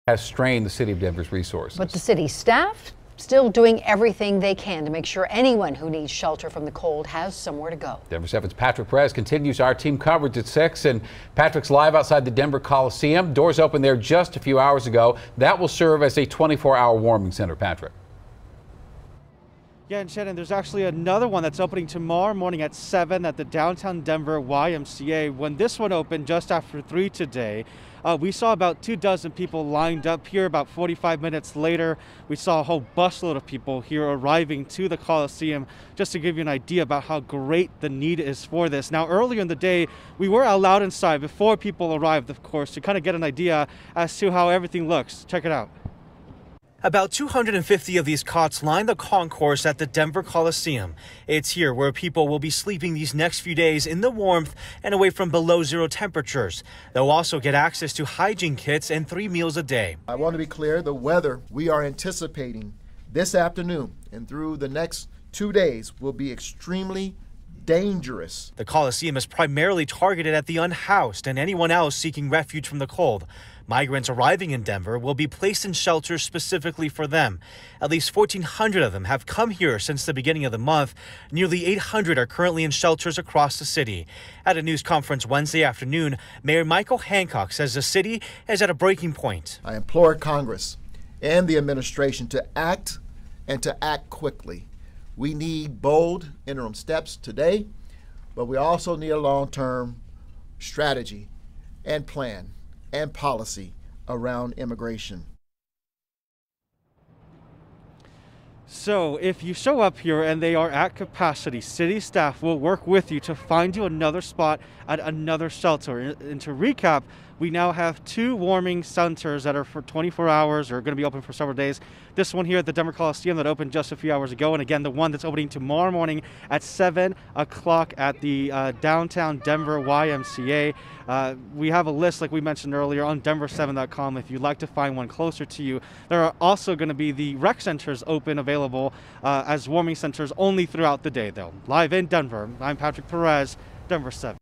Arctic blast, surge of migrants strain resources in Denver to shelter people from conditions
Ahead of an arctic blast beginning Wednesday night, Denver authorities stressed their work to encourage people experiencing homelessness to seek shelter, as the city continues to provide resources for the more than 1,300 migrants who have arrived in the city this month.